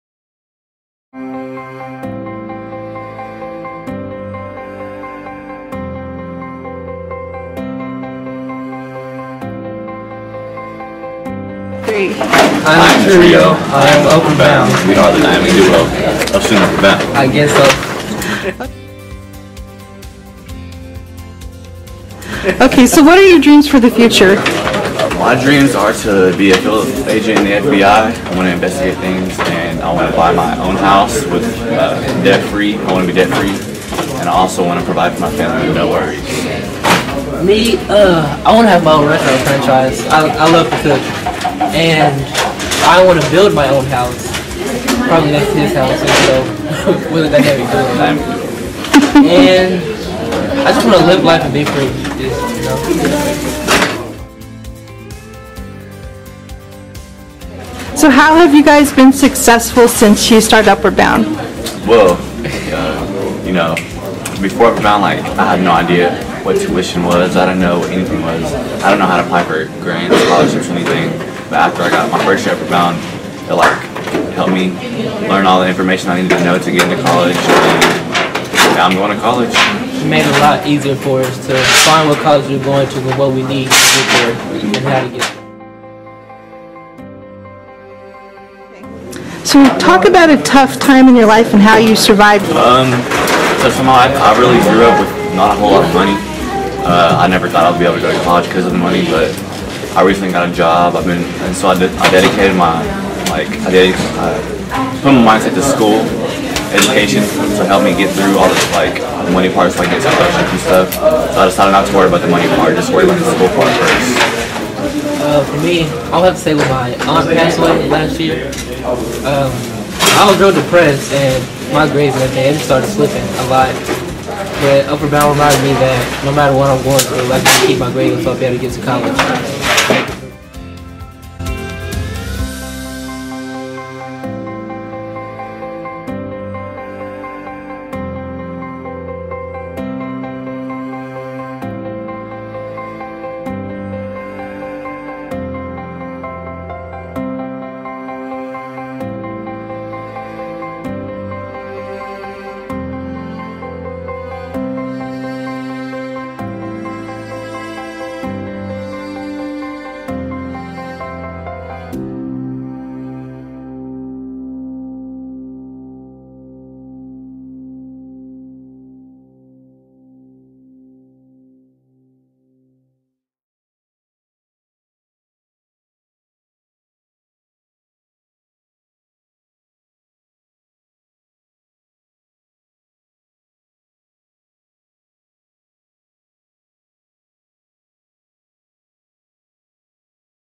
I AM Upward Bound - University of Oklahoma Upward Bound 2017
A short interview with 2 Trio students about their experiences with Upward bound.
(Audio has some issues with background noise)